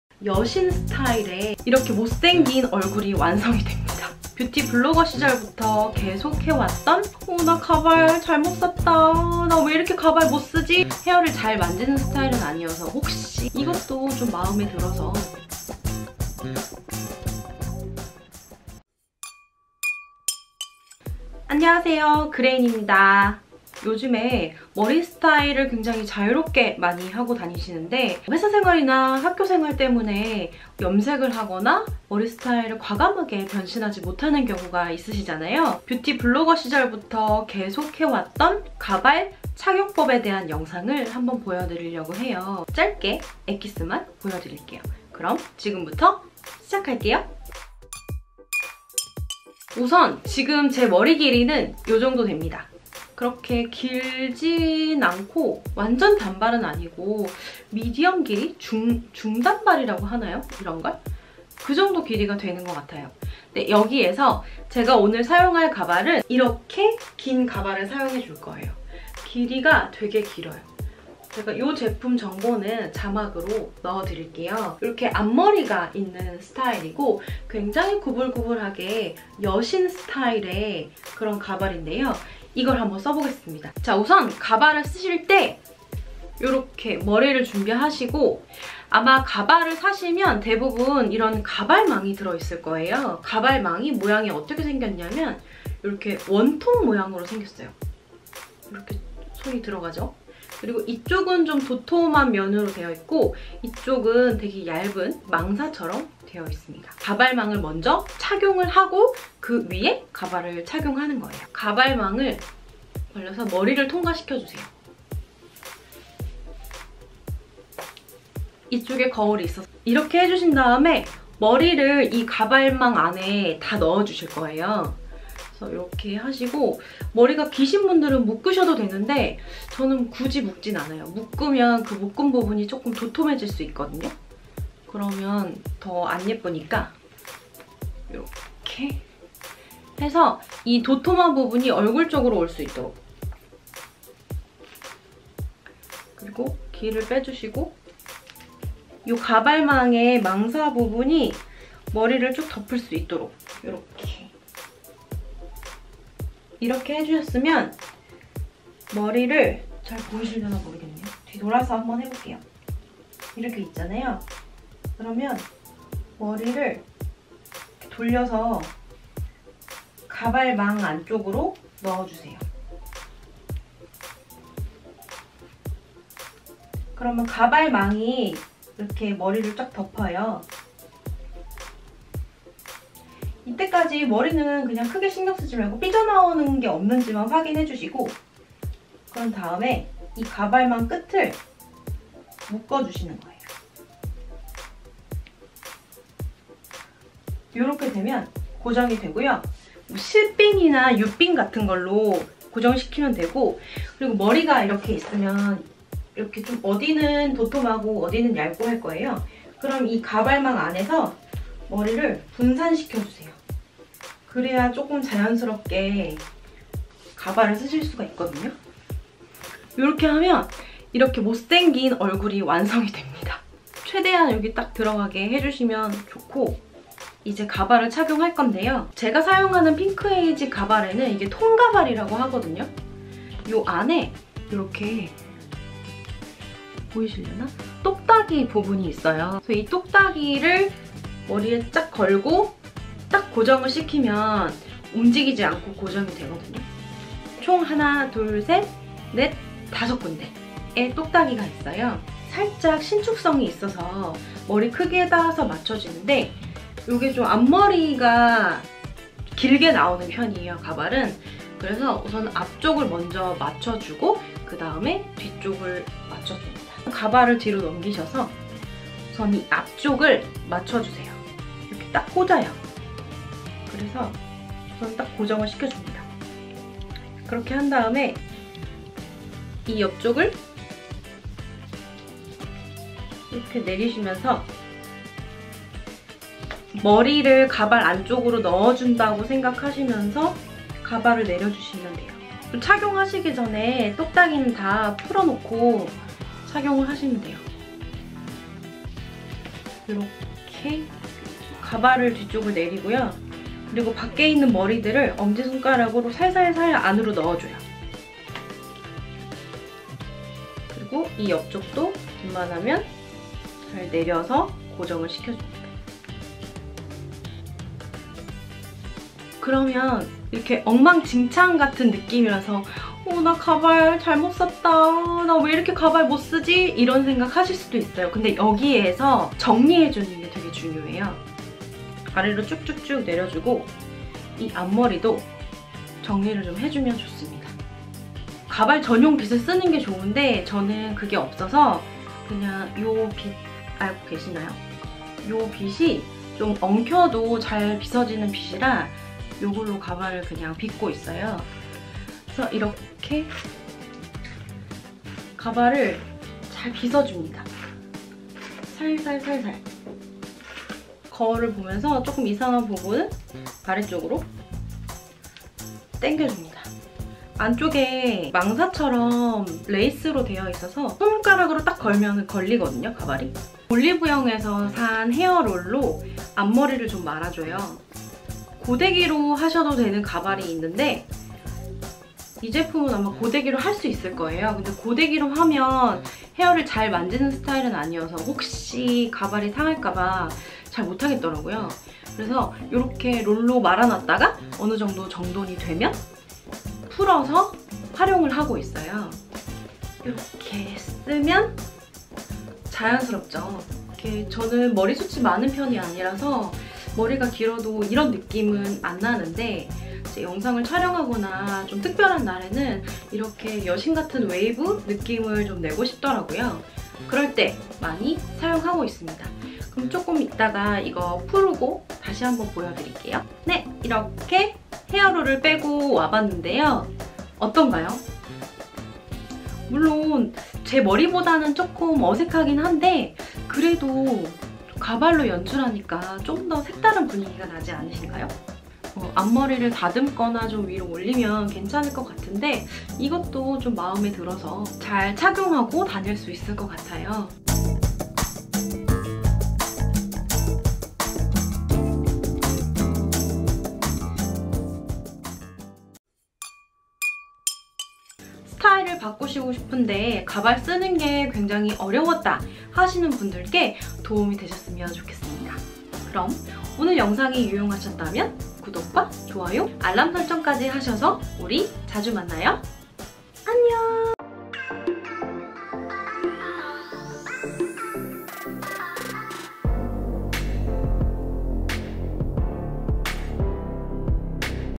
가발쓰는법🧒🏻-👩🏻통가발 자연스럽게 쓰는 노하우 / 핑크에이지 / 긴머리헤어스타일 / 웨이브펌 / 여신머리
안녕하세요^^그레인이에요!

오늘은 스타일을 확 바꿀 수 있는
가발쓰는법에 대한 영상을 가져왔어요.

뷰티블로그를 하면서 가발을 자주 썼던 저 만의 간단한 방법이 있거든요.
생각보다 어렵지 않으니까 같이 가발 쓰고 스타일 변신해봐요.

오늘도 제 영상 보러 와주셔서 감사합니다^^
구독(은 무료니까 맘껏!)과 좋아요 그리고 알람 전체 설정 까지 하셔서
우리 자주 만나요!

그럼 빠잉!